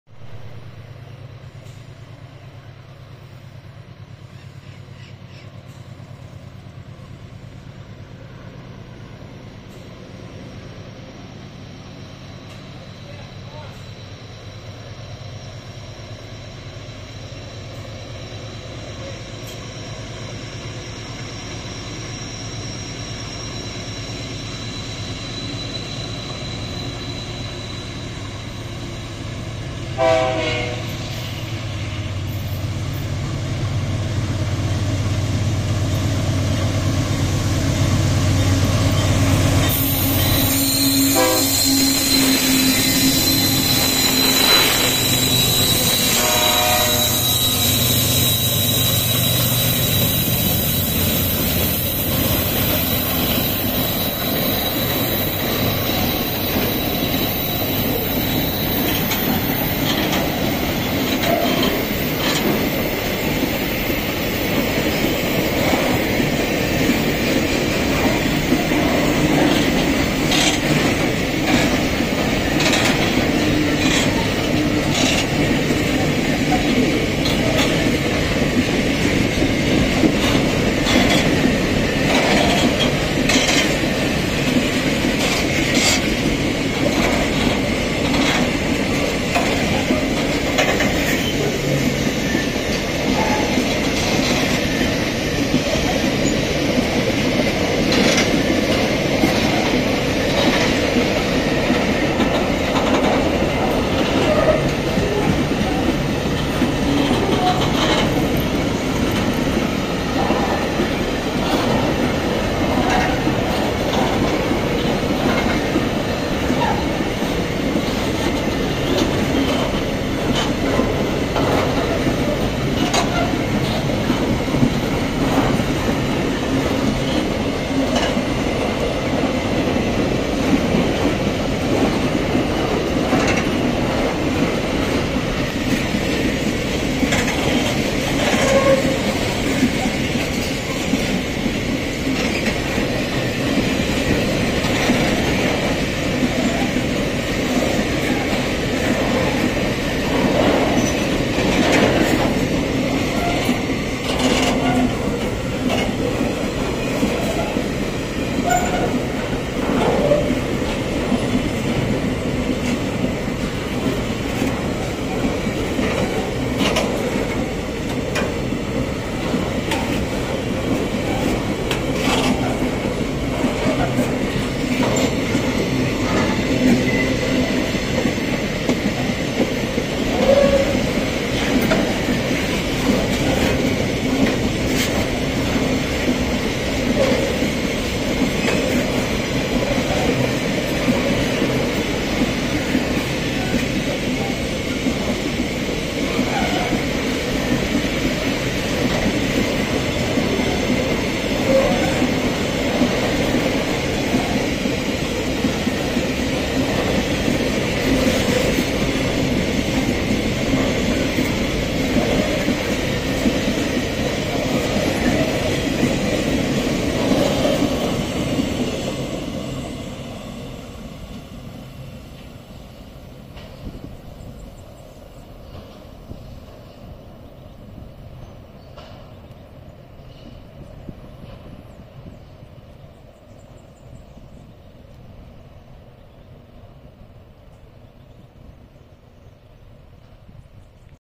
Trem da MRS na linha do centro pátio do 315
trem da MRS circulando na ferrovia do centro na altura do pátio do km 315 sentido Santos Dumont Barbacena Jeceaba Minas gerais esse trem teve origem no estado do Rio de janeiro